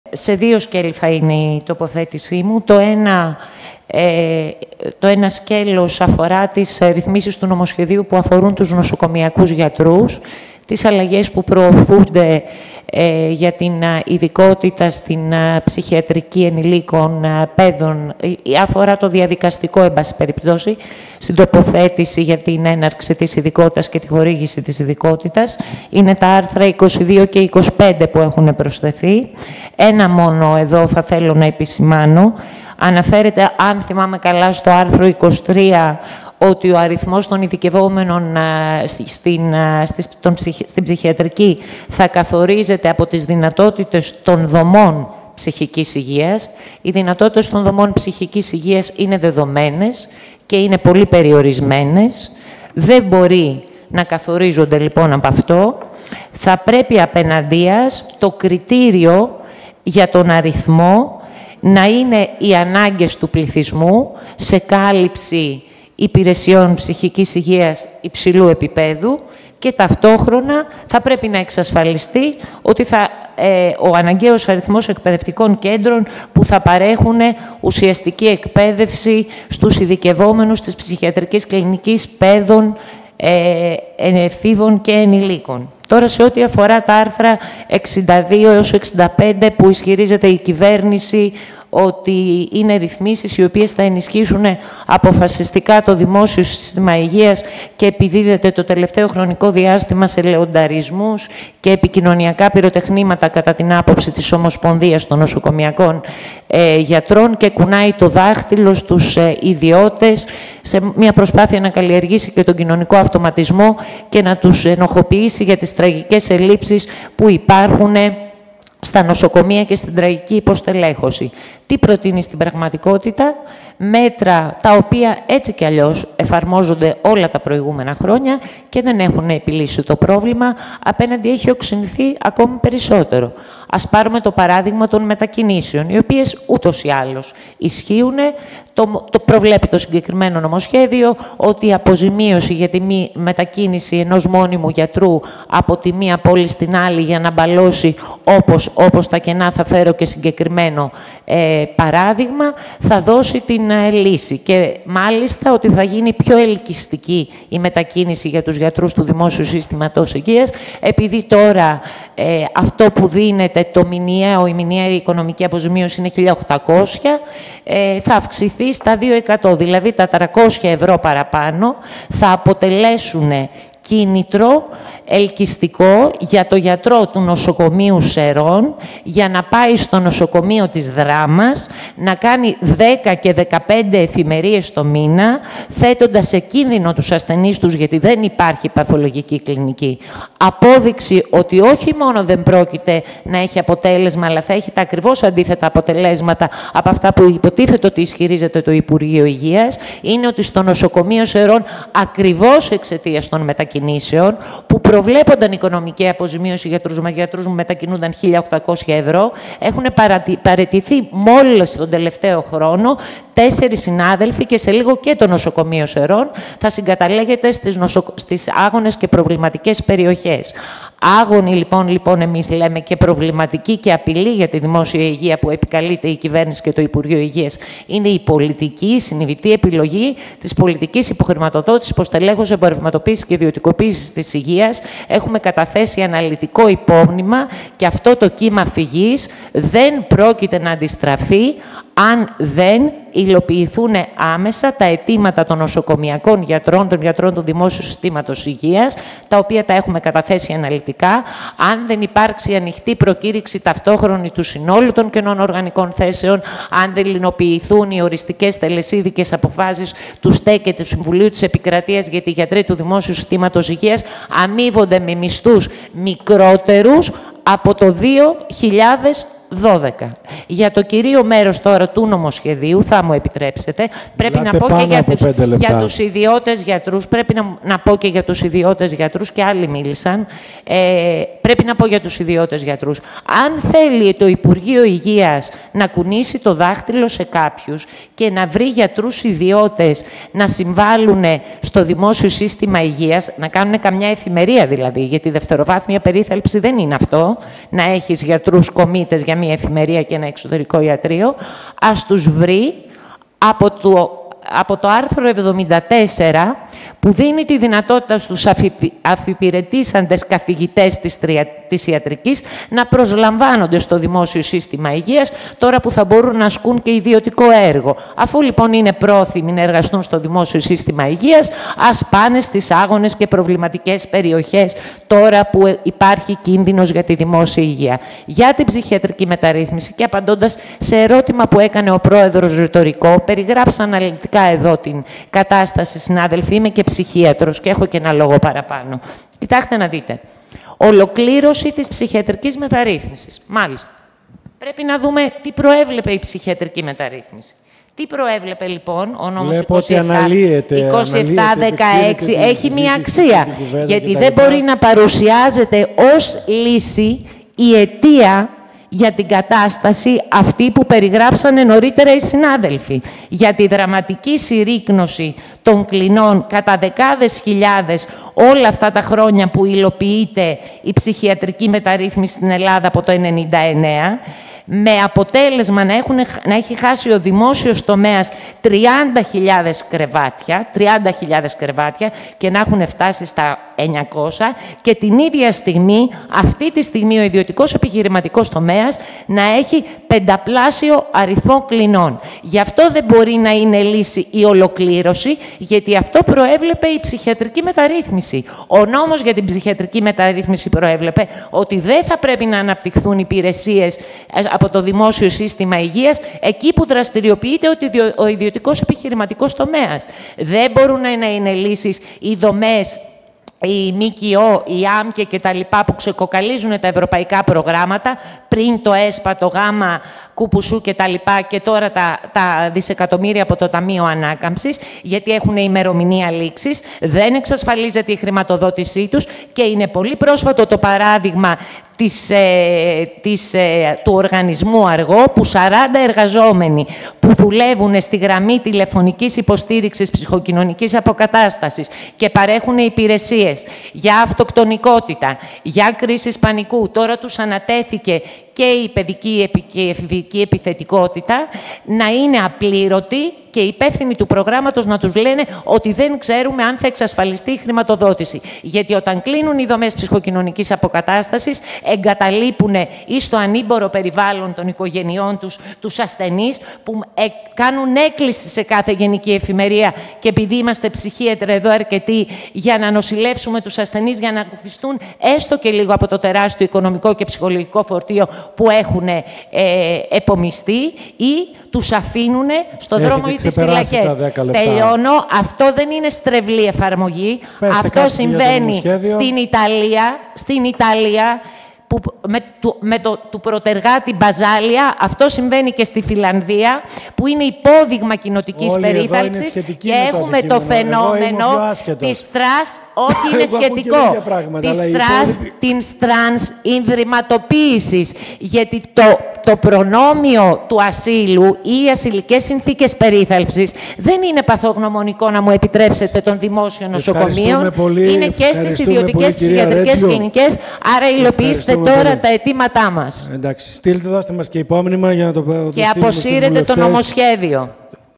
Η Αφ. Ρέτζιου στη συζήτηση για το νομοσχέδιο για την ψυχική υγεία και την απεξάρτηση
Υλοποιήστε τα αιτήματά μας και αποσύρετε το νομοσχέδιο απαίτησε η *Αφροδίτη Ρέτζιου,* πρόεδρος της Ομοσπονδίας Ενώσεων Νοσοκομειακών Γιατρών Ελλάδας (ΟΕΝΓΕ).

Σημείωσε πως με το άρθρο 23, ο αριθμός των ειδικευόμενων θα καθορίζεται από τις δυνατότητες των δομών, που είναι δεδομένες και περιορισμένες. Αντιθέτως, δεν μπορεί να καθορίζεται από αυτό, αλλά το κριτήριο πρέπει «να είναι οι ανάγκες του πληθυσμού σε κάλυψη υπηρεσιών ψυχικής υγείας υψηλού επιπέδου» και ταυτόχρονα θα πρέπει να εξασφαλιστεί ο αναγκαίος αριθμός εκπαιδευτικών κέντρων που θα παρέχουν ουσιαστική εκπαίδευση στους ειδικευόμενους της ψυχιατρικής κλινικής παίδων, εφήβων και ενηλίκων.

Τόνισε πως υπάρχουν μέτρα που εφαρμόζονται όλα τα προηγούμενα χρόνια και δεν έχουν επιλύσει το πρόβλημα, αλλά οξύνθηκε ακόμα περισσότερο, φέρνοντας ως παράδειγμα τις μετακινήσεις γιατρών προκειμένου να «μπαλωθούν» όπως - όπως τα κενά.  Απόδειξη πως θα υπάρχουν τα ακριβώς αντίθετα αποτελέσματα, είναι ότι στο νοσοκομείο των Σερρών τέσσερις γιατροί τον τελευταίο χρόνο έχουν παραιτηθεί και σύντομα θα συγκαταλέγεται στις άγονες και προβληματικές περιοχές. «Άγονη και προβληματική και απειλή για τη δημόσια υγεία, είναι η πολιτική, συνειδητή επιλογή της υποχρηματοδότησης, υποστελέχωσης, εμπορευματοποίησης και ιδιωτικοποίησης της υγείας και αυτό το κύμα φυγής δεν πρόκειται να αντιστραφεί αν δεν υλοποιηθούν άμεσα τα αιτήματα των νοσοκομειακών γιατρών, των γιατρών του δημόσιου συστήματος Υγείας, αν δεν υπάρξει ανοικτή ταυτόχρονη προκήρυξη του συνόλου των κενών οργανικών θέσεων, αν δεν υλοποιηθούν οι οριστικές, τελεσίδικες αποφάσεις του ΣτΕ γιατί οι γιατροί του δημόσιου συστήματος Υγείας αμείβονται με μισθούς μικρότερους από το 2012», ξεκαθάρισε.